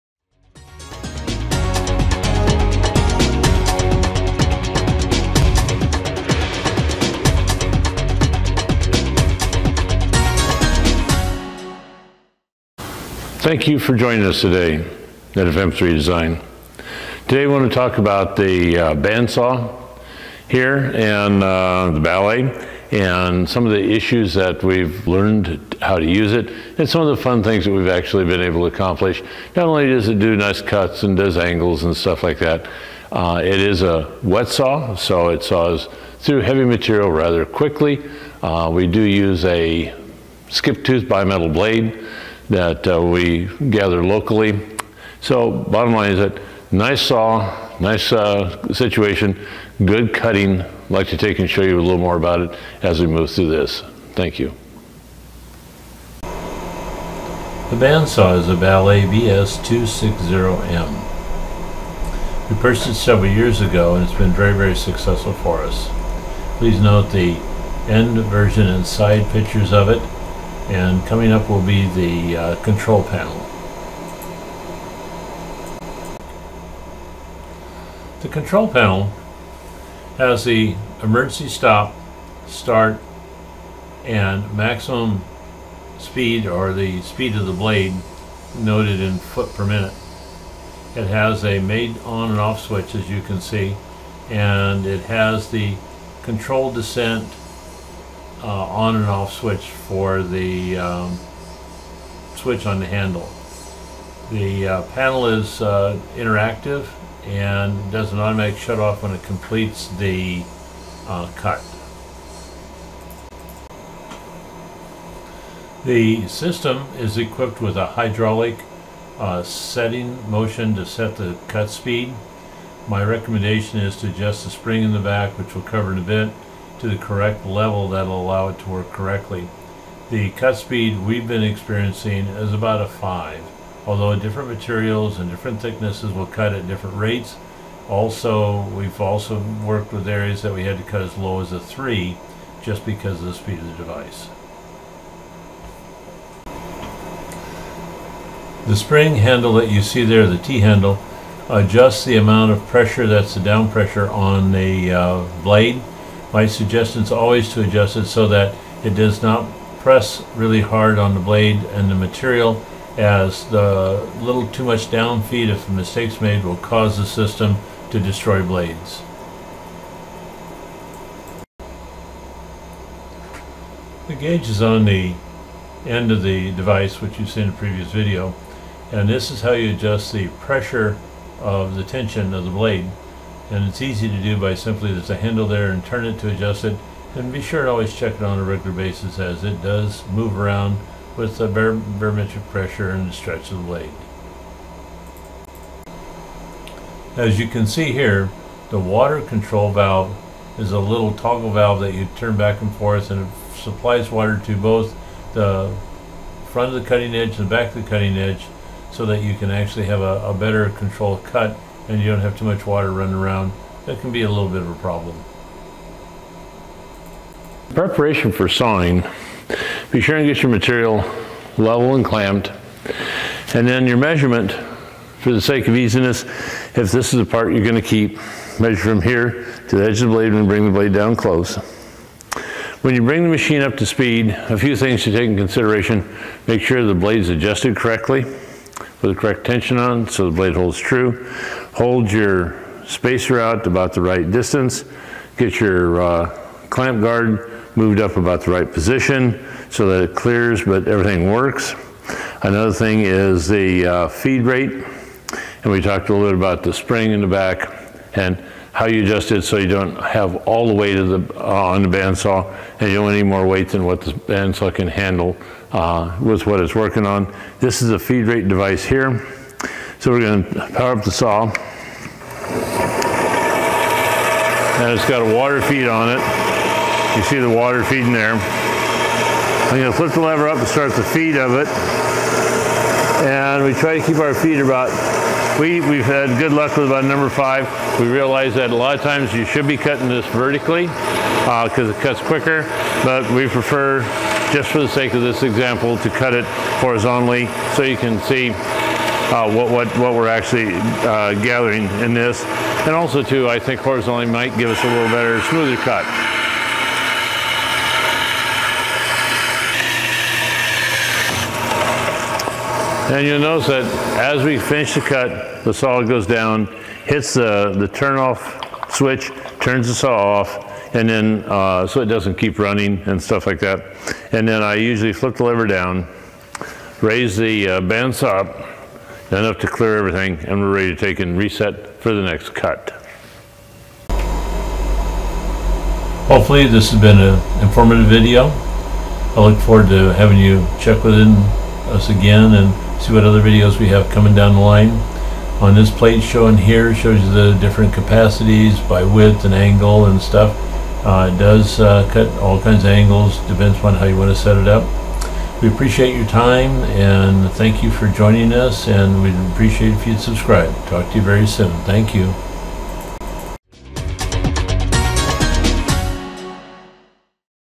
Baileigh model BS250M band saw
Baileigh horizontal band saw - model BS250M - a walk thru the operation of the saw and explanation of how it works and some settings